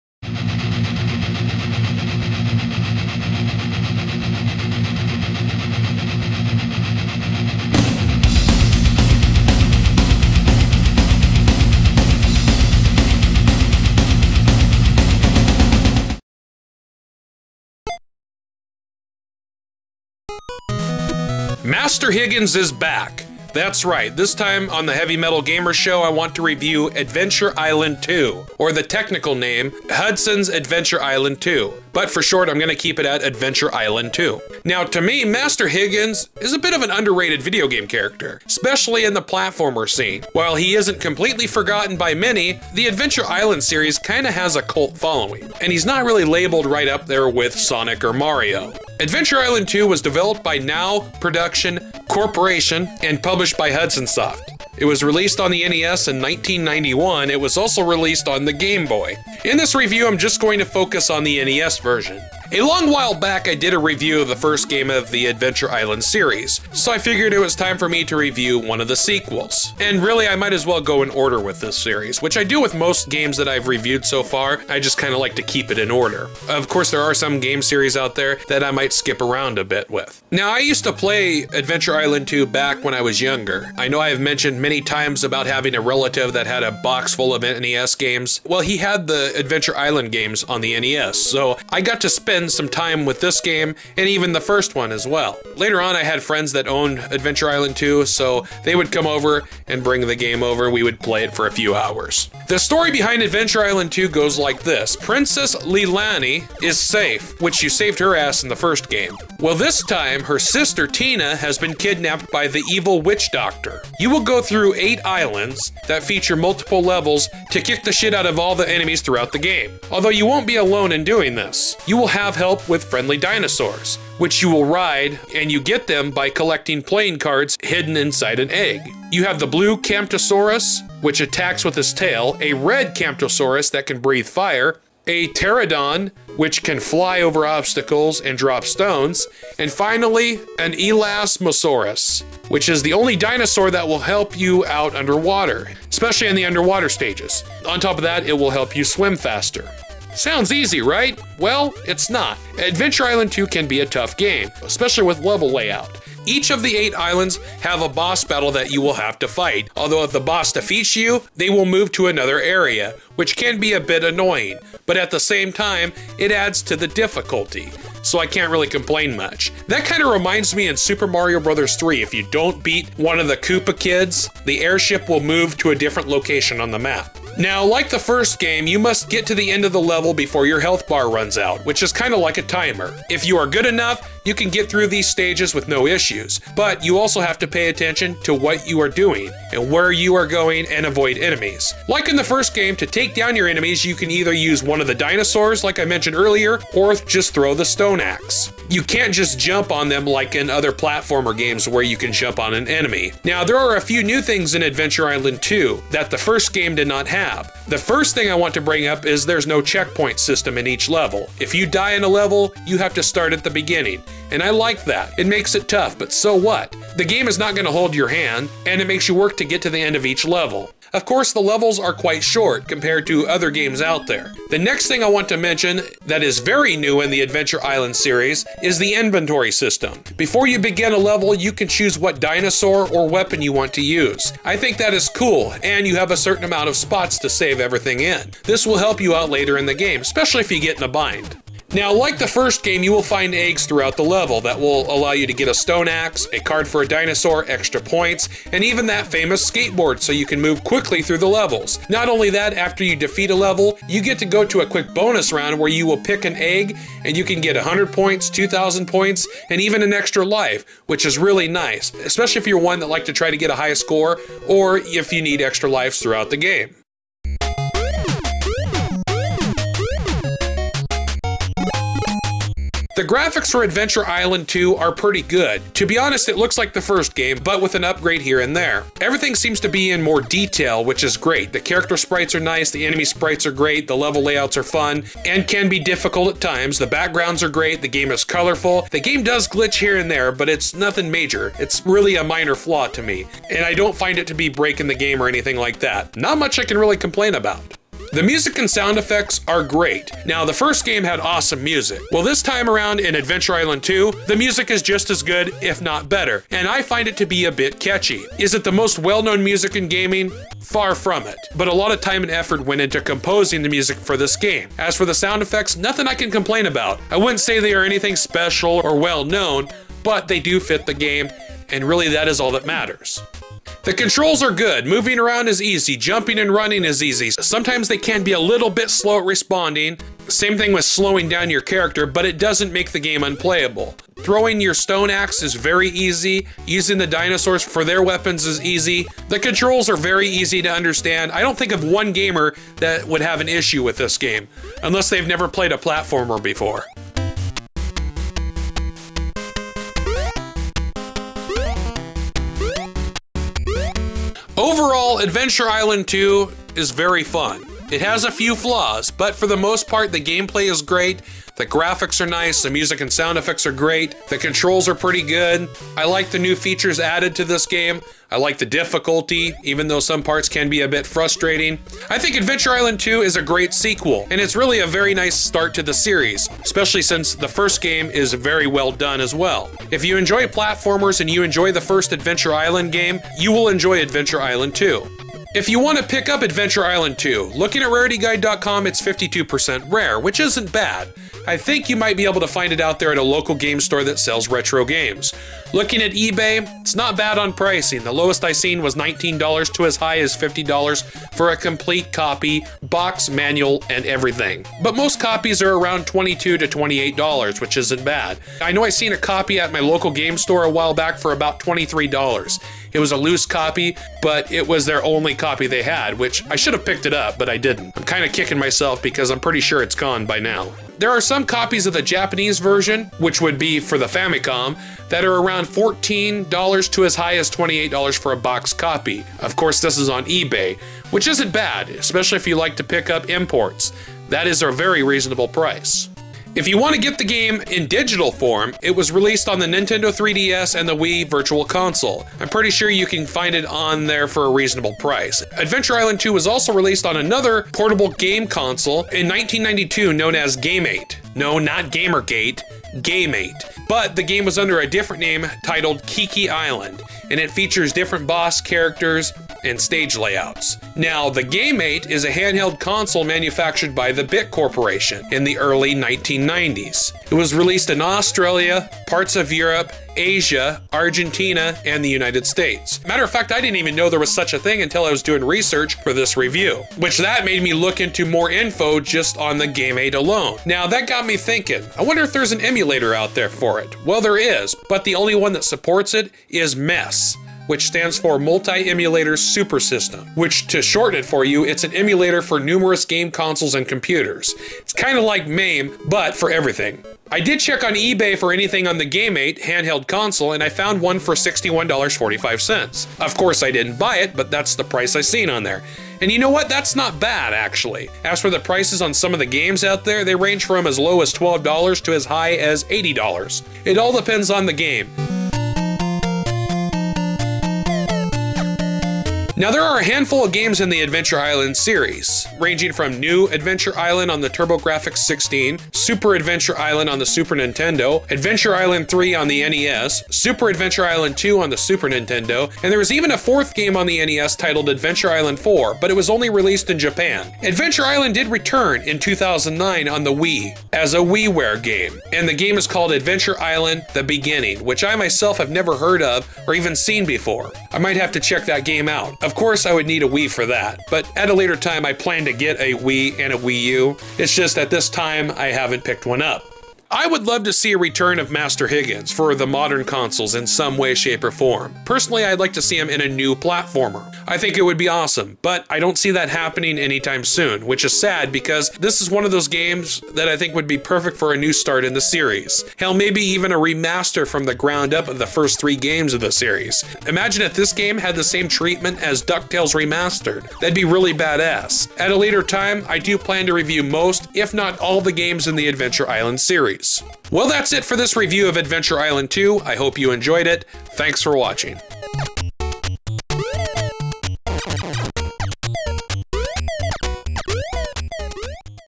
Heavy Metal Gamer: Adventure Island II Review
It's the start of another week, so I figure it's time to check out Adventure Island II, also known as Hudson's Adventure Island II. I did a review of the first game a long time ago, and I figure it was time for a revew of the sequel. I hope you enjoy this review.